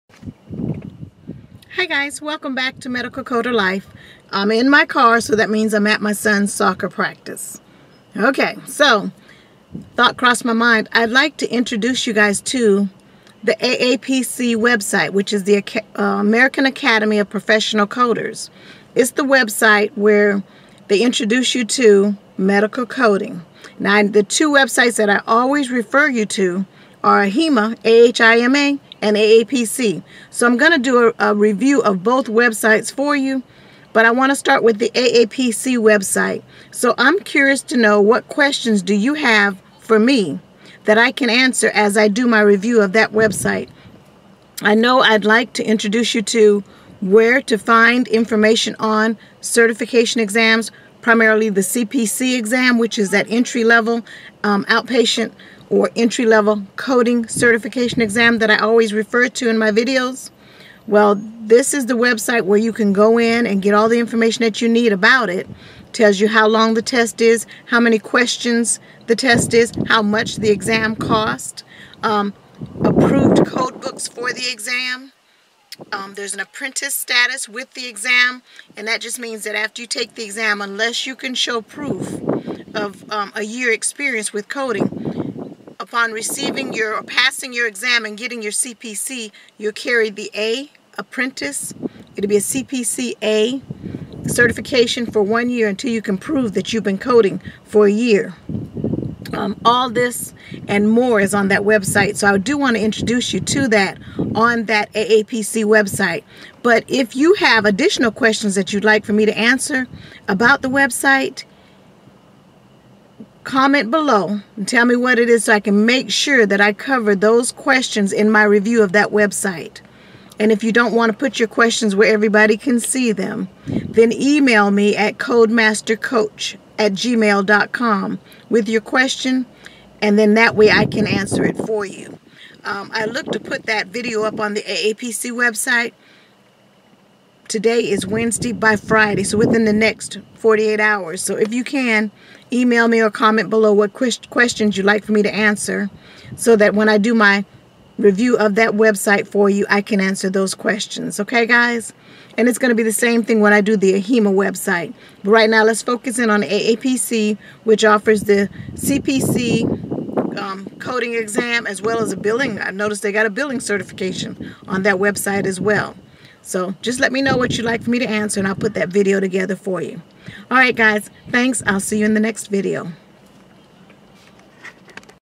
AAPC Website: Questions
I would like to introduce you to the AAPC website.  This website will answer your questions about coding and certification.  Do  you have questions you'd like for me to make sure I answer with this review of the website?